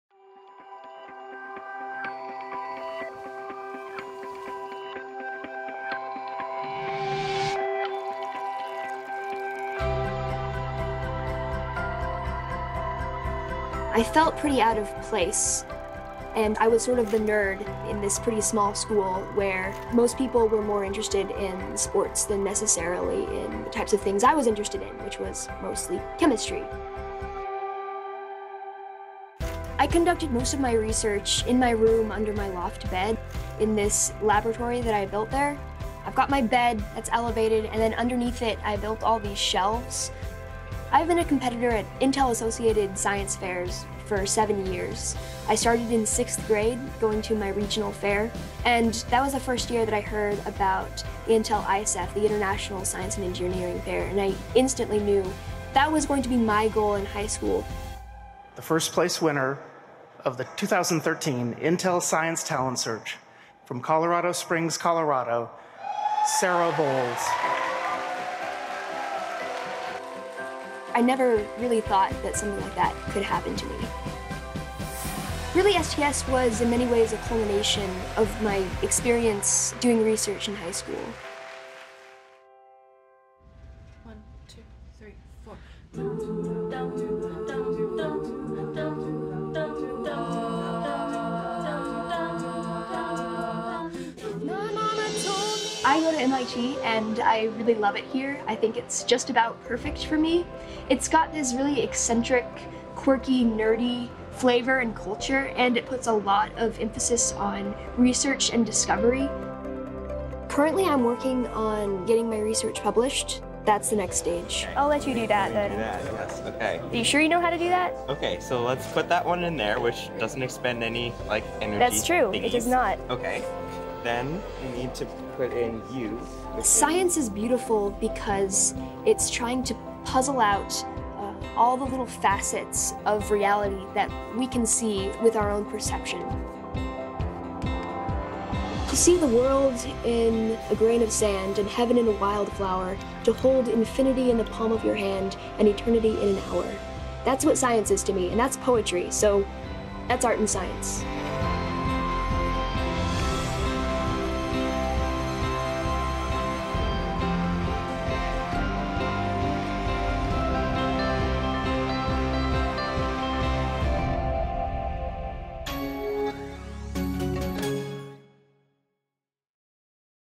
Algae Girl, One Year After 2013 Intel STS Win
Sara Volz' path to Intel STS 2013 and beyond.